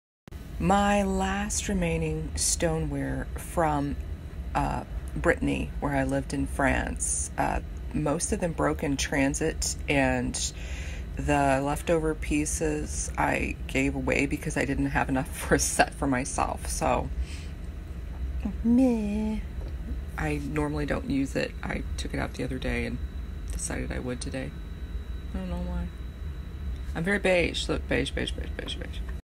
Stoneware from Bretagne, France
It was purchased new by Moi more than 20 years ago.